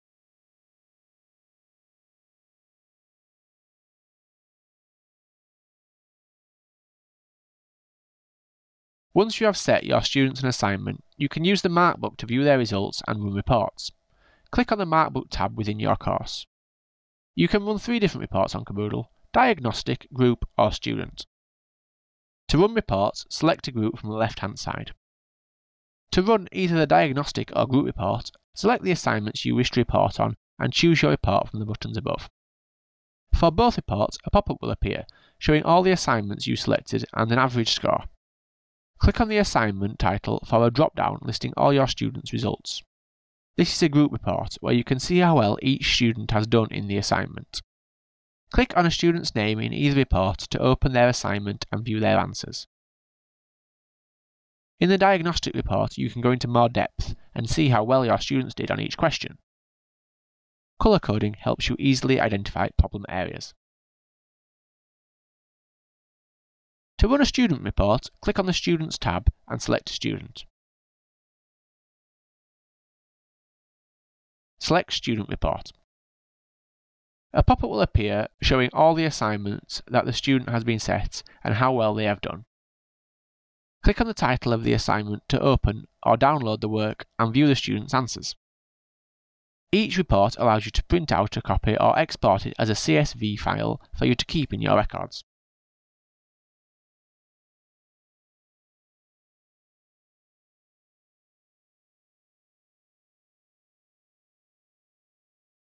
Using the Markbook on Kerboodle
You can run three different reports on Kerboodle to help you track students' progress. Follow these instructions to find out how.

For more support on Kerboodle, including a bank of FAQs, with our support